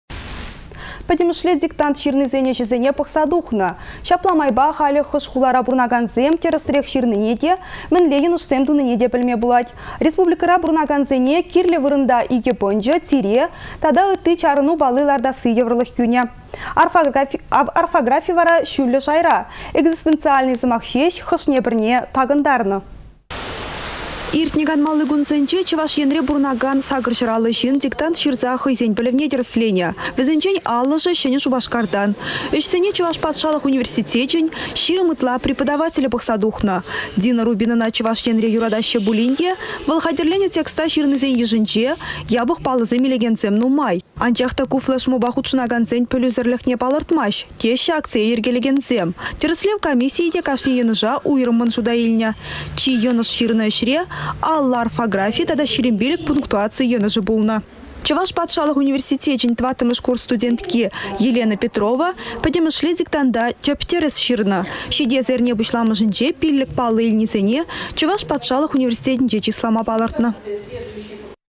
«Пĕтĕмĕшле диктанта» пĕтĕмлетнĕ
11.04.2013
Пĕтĕмĕшле диктант çырнисен ĕçĕсене пăхса тухнă. Çапла майпа халĕ хăш хулара пурăнакансем тĕрĕсрех çырнине те, мĕнле йăнăшсем тунине те пĕлме  пулать. Республикăра пурăнакансене кирлĕ вырăнта икĕ пăнчă, тире тата ытти чарăну палли лартасси йывăрлăх кÿнĕ. Чăваш Енре орфографи çÿллĕ шайра - «экзистениальный» сăмах çеç хăшне-пĕрне такăнтарнă.